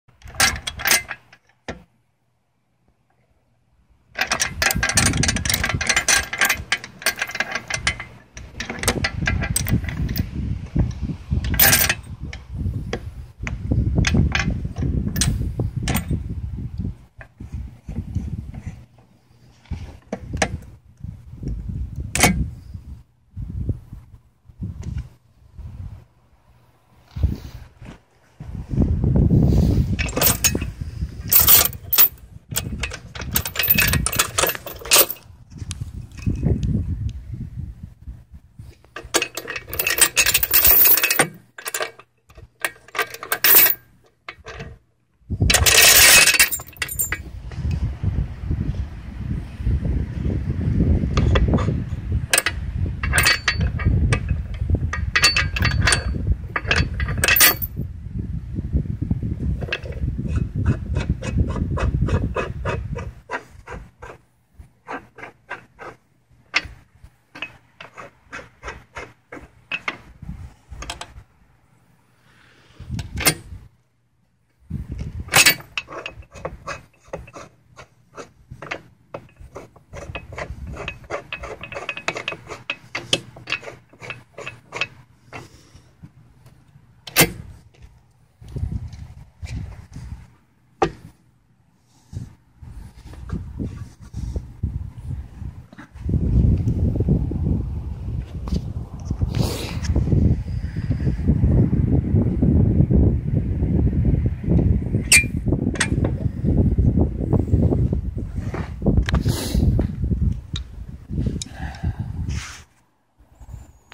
20200602 swing blade mill dogs adjustment
Recently acquired one of the DL Timber Tech 180º swing blade mills. Just learning how to dog the logs. A bit slow at it, first time, figuring it all out and only using one hand (recording video with the other). Next up, start the engine and make some sawdust.